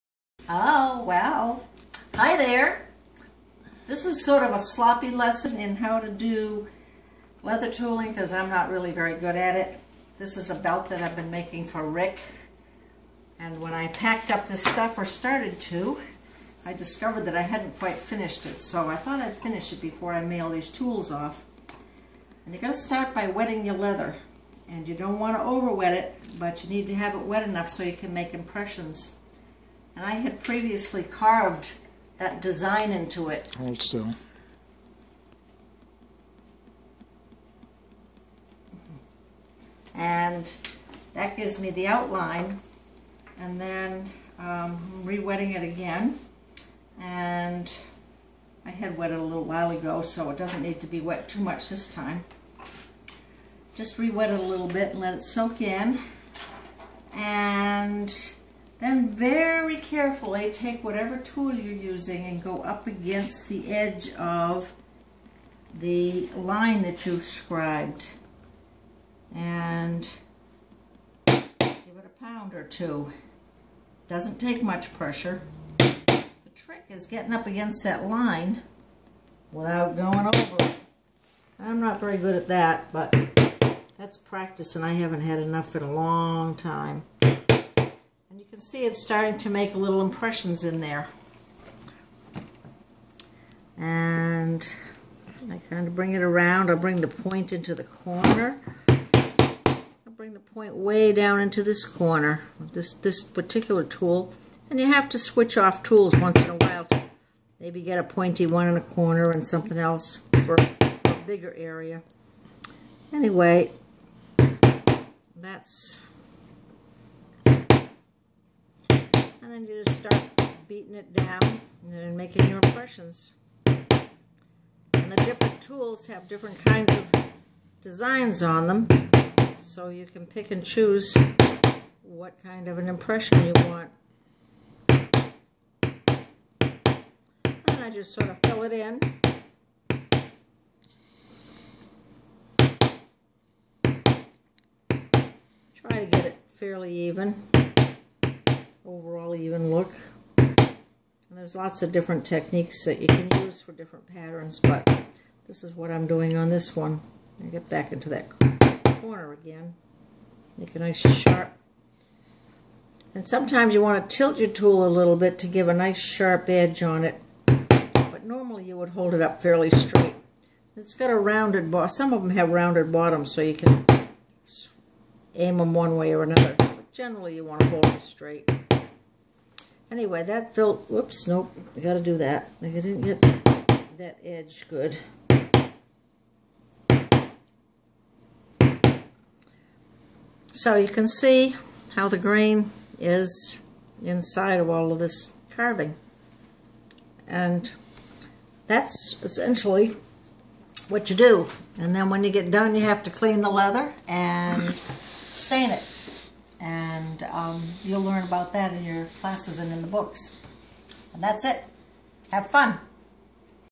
131218 Trudy Leather Working
A quickie lesson for the kids on leather working.